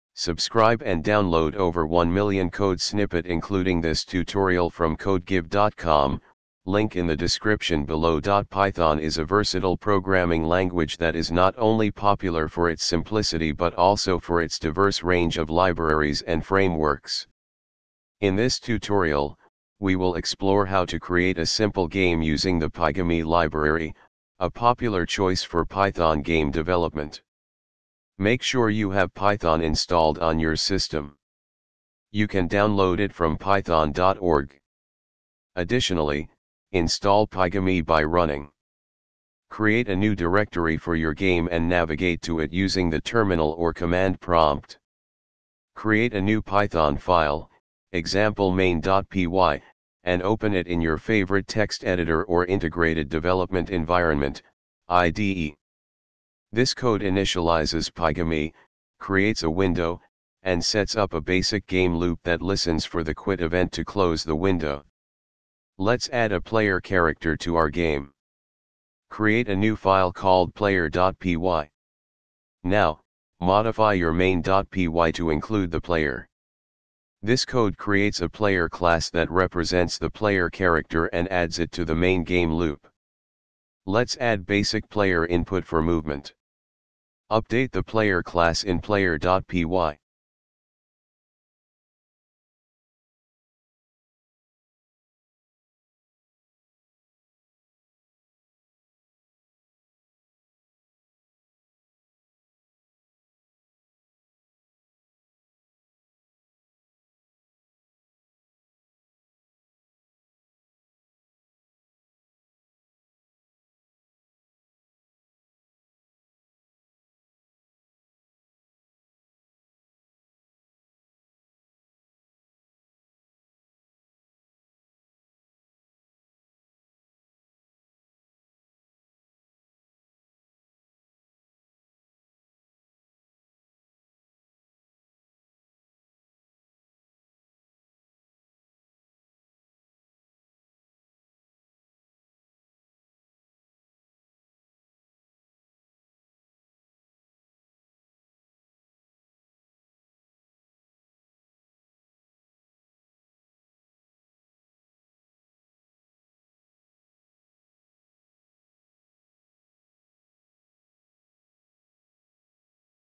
games in python
Python is a versatile programming language that is not only popular for its simplicity but also for its diverse range of libraries and frameworks. In this tutorial, we will explore how to create a simple game using the Pygame library, a popular choice for Python game development.
Make sure you have Python installed on your system. You can download it from python.org. Additionally, install Pygame by running:
Create a new directory for your game and navigate to it using the terminal or command prompt.
Create a new Python file, e.g., main.py, and open it in your favorite text editor or integrated development environment (IDE).
This code initializes Pygame, creates a window, and sets up a basic game loop that listens for the quit event to close the window.
Let's add a player character to our game. Create a new file called player.py:
Now, modify your main.py to include the player:
This code creates a Player class that represents the player character and adds it to the main game loop.
Let's add basic player input for movement. Update the Player class in player.py: